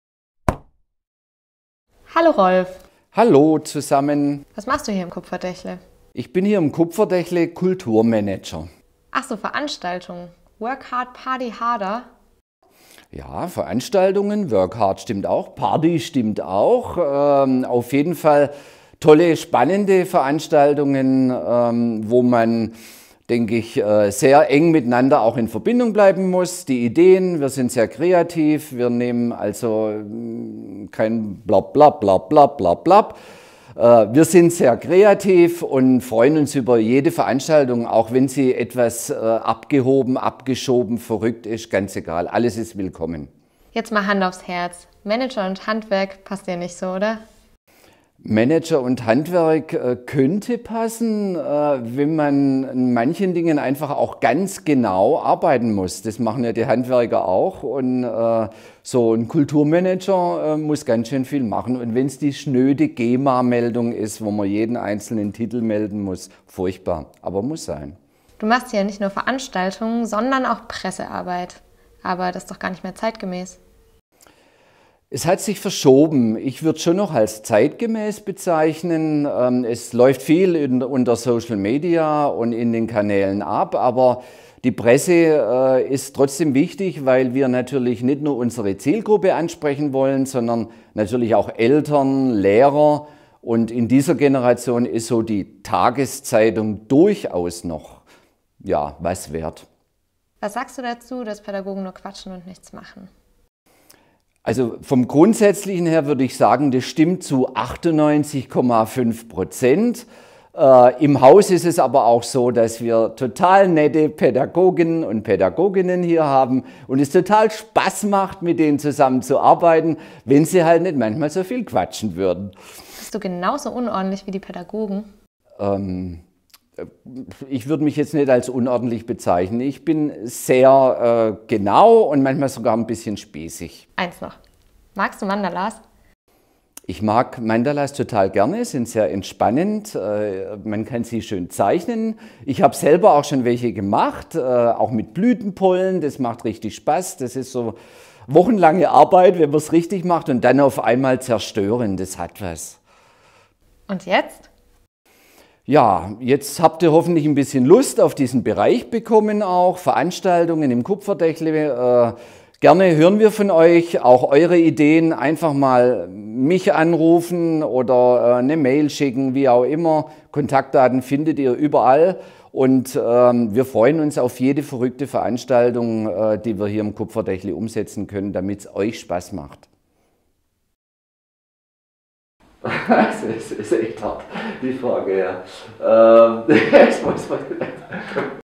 8 Fragen an Rolf | Veranstaltungsbereich | Kupferdächle
Wer treibt sich allgemein denn eigentlich so im Kupferdächle rum?
Wer ist für welchen Bereich zuständig?
Und was macht jeder?

Wir haben Fragen gestellt.

Teil 2 unserer Reihe: Rolf & der Veranstaltungsbereich!

AKTUELL BLEIBEN!